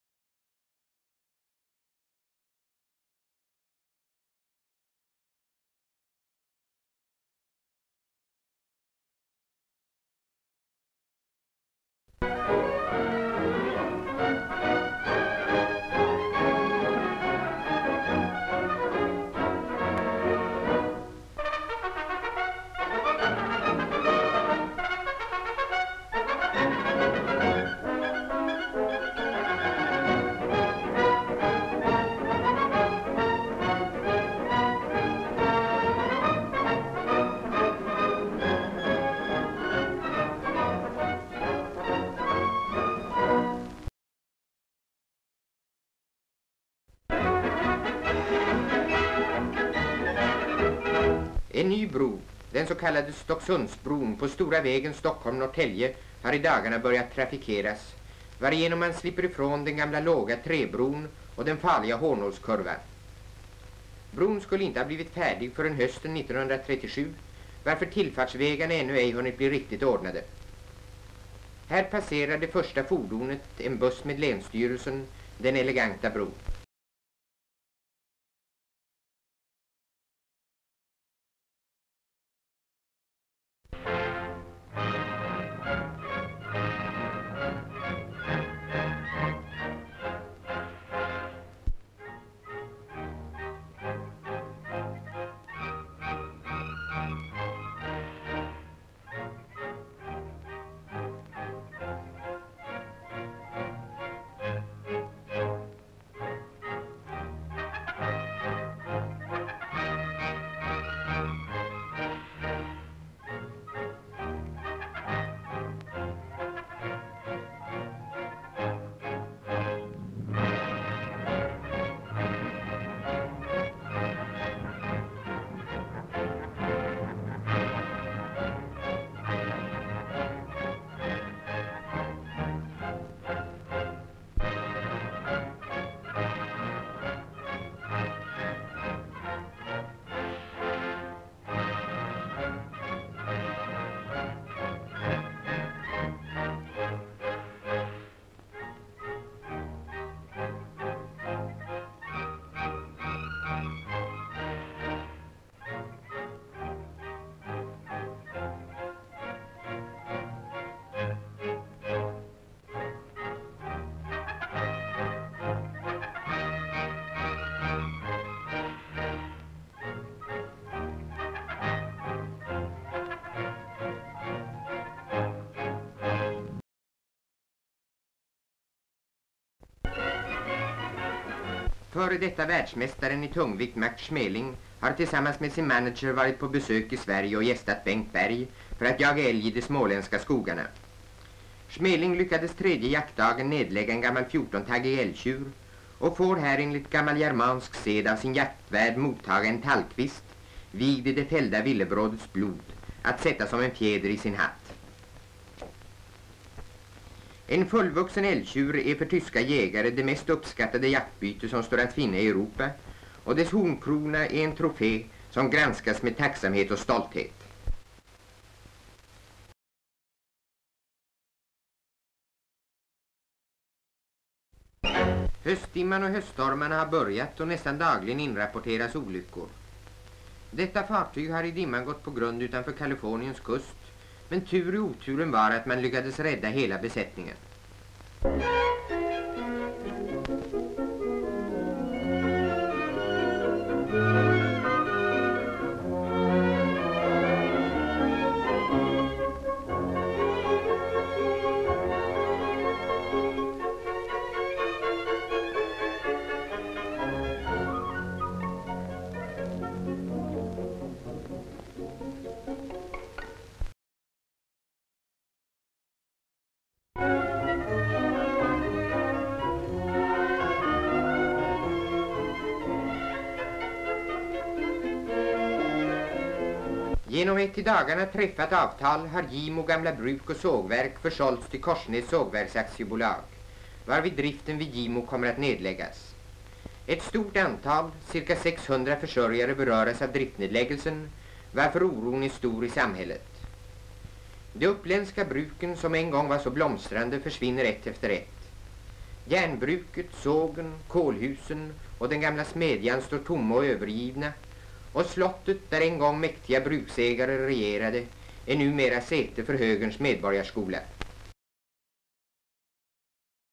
1936-11-02(1936) 1936 närvarade Gustaf Adolf vid avtäckningen av Narvamonumentet i Estland som rest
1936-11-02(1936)

1936 närvarade Gustaf Adolf vid avtäckningen av Narvamonumentet i Estland som restes till minne av de stupade vid slaget vid Narva 19 november 1700 som utkämpades mellan Karl XII och tsar Peter I:s styrkor. Statyn var en kopia av Slottslejonet vid Stockholms slott och totalförstördes under Andra världskriget. Ett nytt monument avtäcktes av vice statsminister Lena Hjelm-Wallén den 19 november 2000 i samband med 300-årsminnet av slaget vid Narva som en symbol för dagens fredliga samarbete mellan Estland och Sverige.

Journalfilmen innehåller även inslagen: Ny riksmarskalk. Stocksundsbron färdig. Max Schmeling jagar älg hos Bengt Berg. Strandad ångare vid klippig kust i Kalifornien. Gimo bruk - en tragisk idyll.

↑ Visa mindre

Hitta filmer via kartan nedan ↓

KategoriJournalfilm

TemaSvensk emigration: Estland

OrtGimo, Kalifornien (USA), Narva, Stockholm, Stocksund

Längd5 minuter

MedverkandeAxel Vennersten, prins Gustav Adolf, Johan Laidoner, Max Schmeling

RättigheterSVT

ProduktionsbolagAB Svensk Filmindustri

ProduktionslandSverige

Utgångsmaterial35 mm, normalbild (1,37:1), optisk mono, svartvit